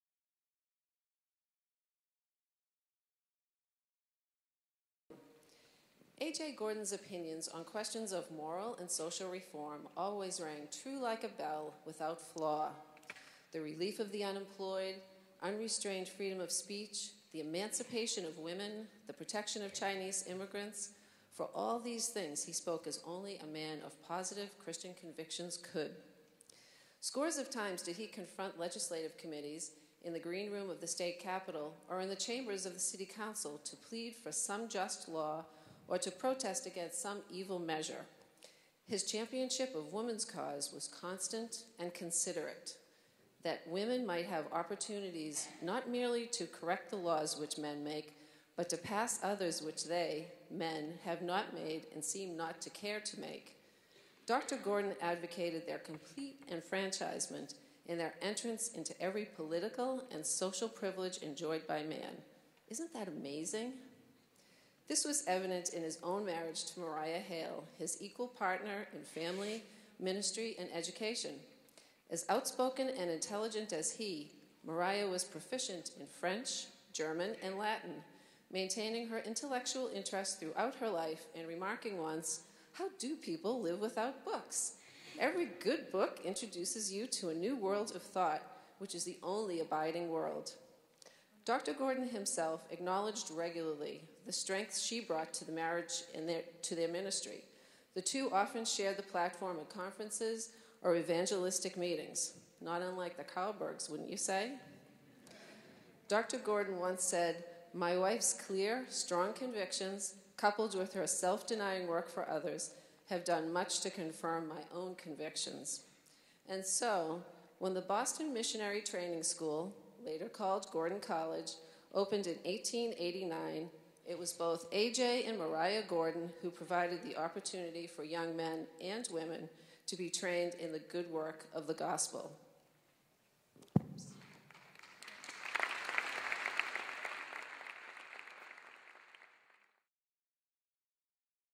255 Grapevine - Uncommon Courage: An A.J. Gordon Story - Sandy Butters
From 255 Grapevine: An Evening With an Address and a Certain Zip - October 9, 2010 - Sandy Butters '93, Vice President for Development